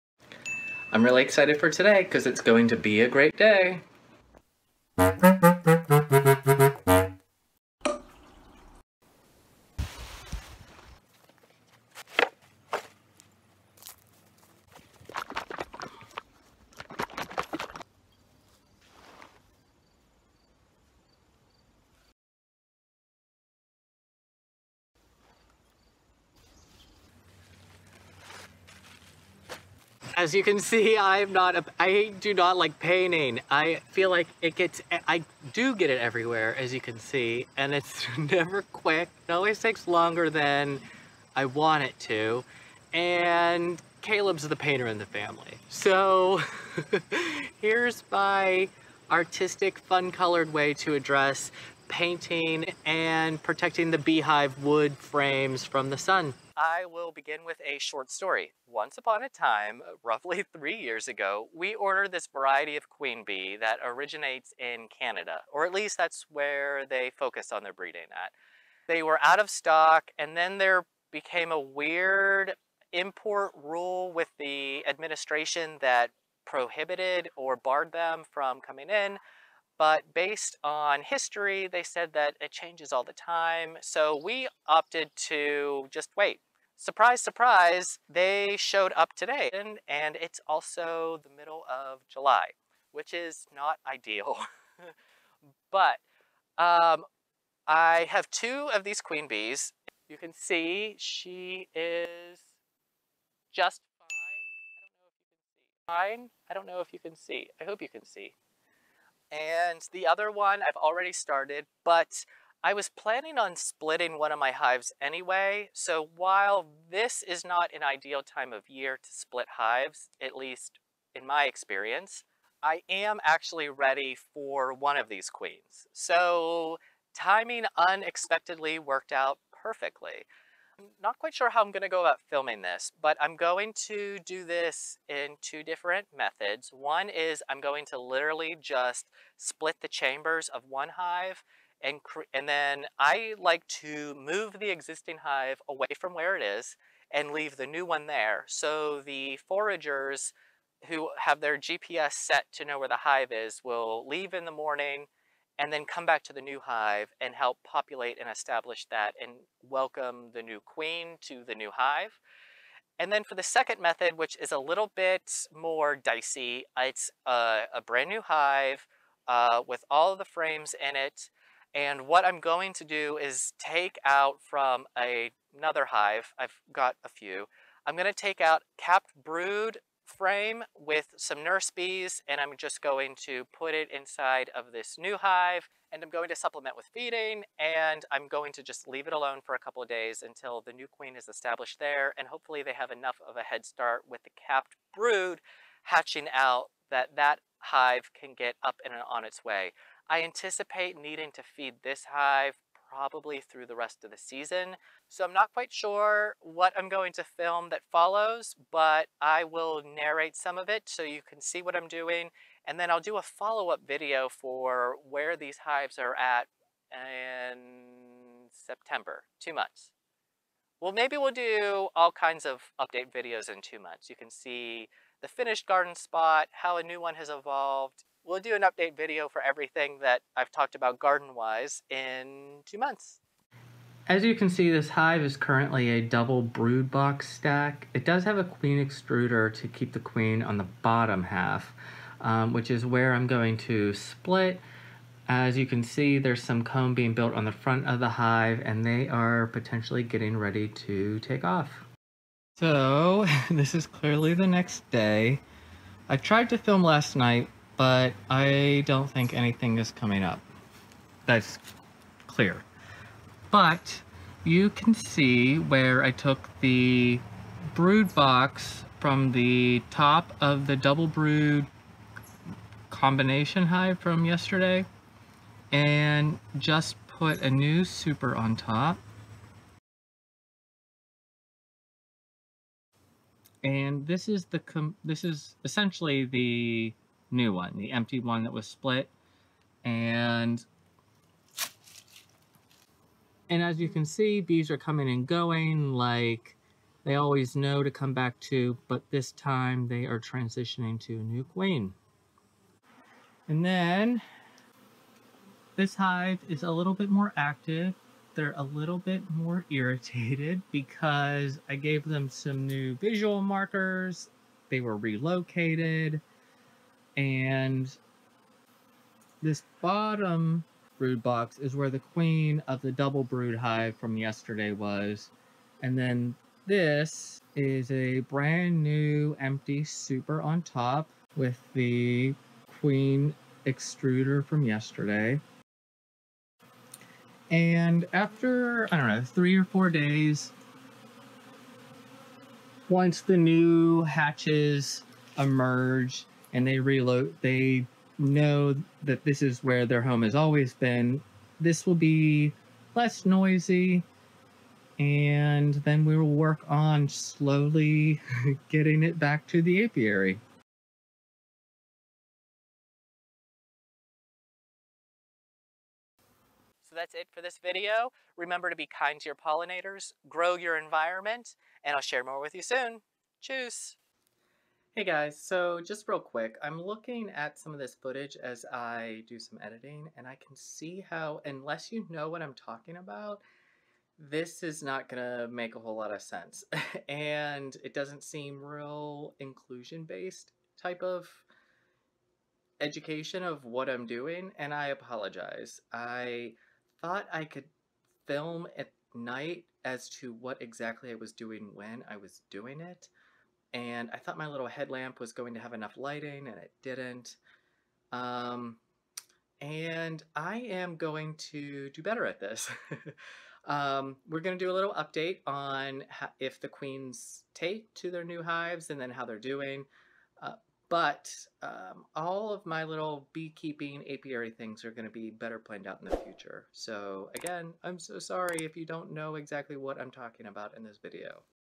My first vlog sharing how to split a honey bee hive.
In this video, as I learn more about editing and filming...I attempt to share one of the things I love most on our property- our honey bees.  So this is what it looks like in one of the many ways you can split a honey bee hive.  Enjoy my first vlog sharing this topic!  (or don't, it's really up to you!  lol)

Take some anxiety out of your life knowing your pets are safe & protected w/ Pet Insurance